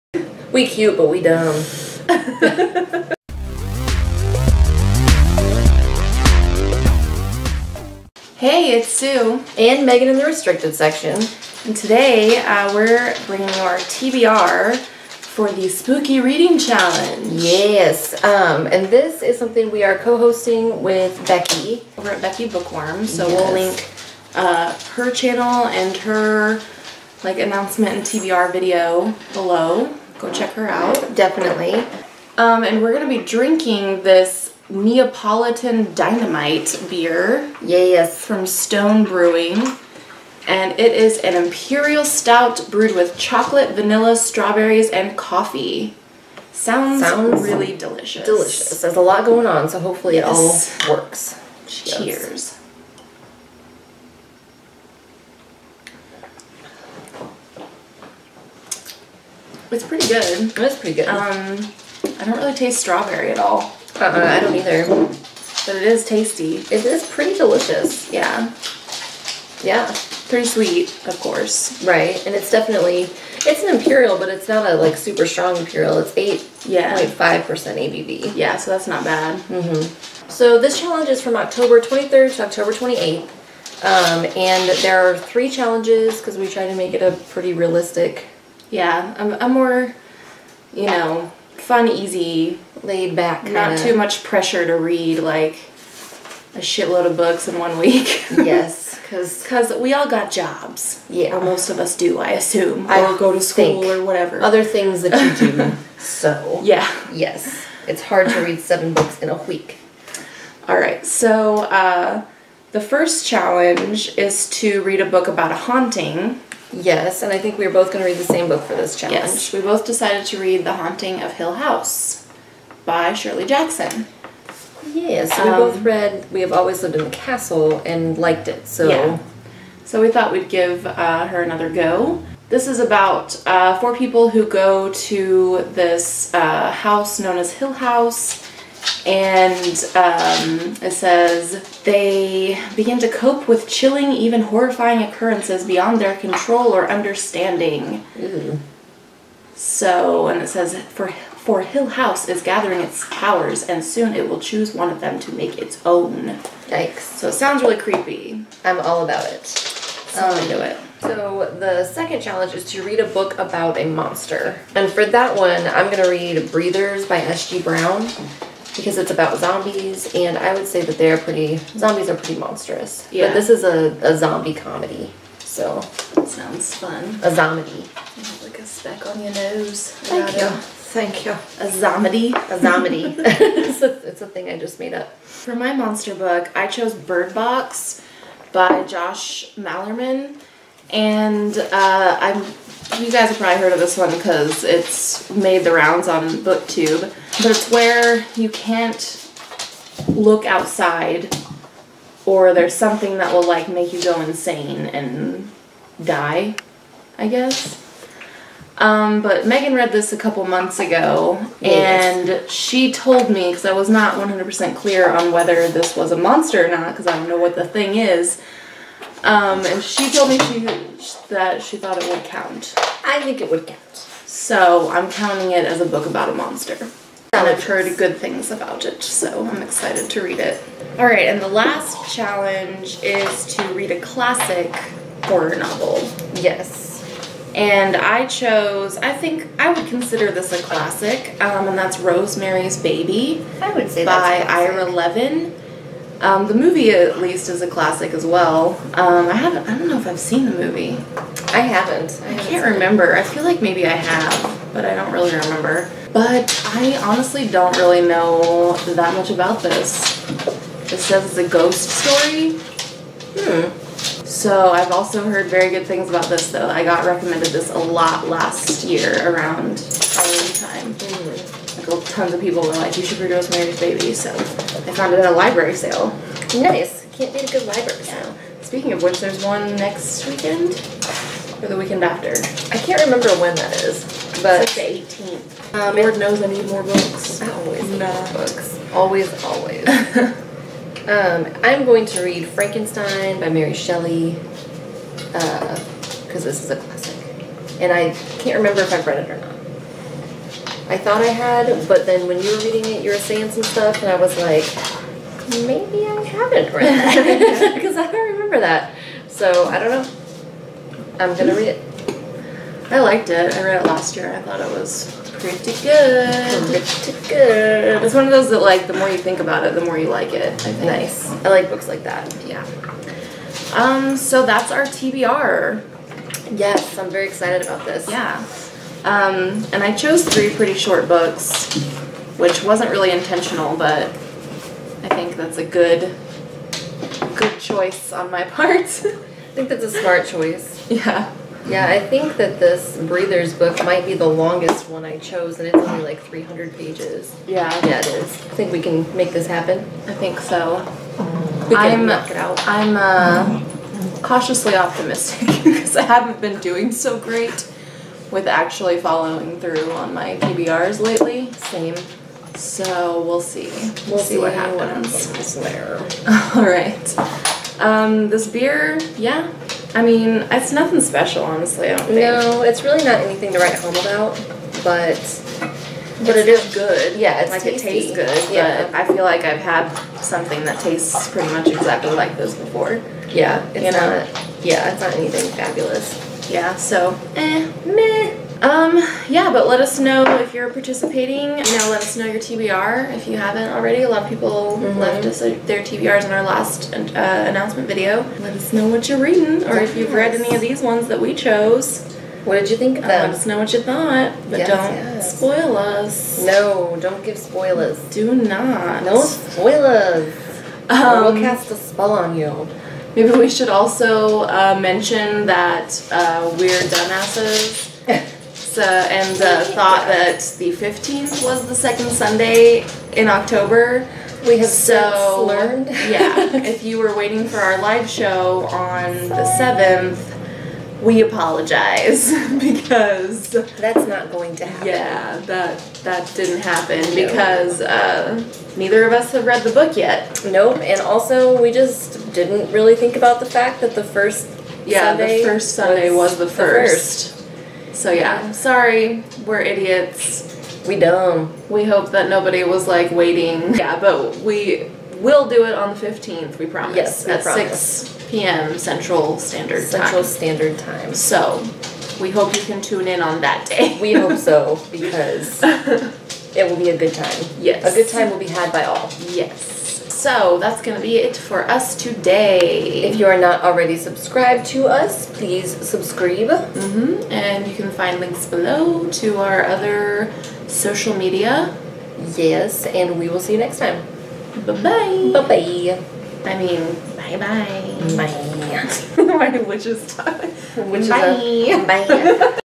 Spooky Reading Challenge TBR
We're drinking some Neapolitan Dynamite by Stone Brewing in collaboration with Abnormal Beer Co. and American Homebrew Association Rally winners Paul Bischeri and Patrick Martinez.

Dates: October 23rd - 28th

The Challenges:
1. Read a book about a haunting
2. Read a book about a monster
3. Read a horror classic

Untappd: meknight

Untappd: SueCee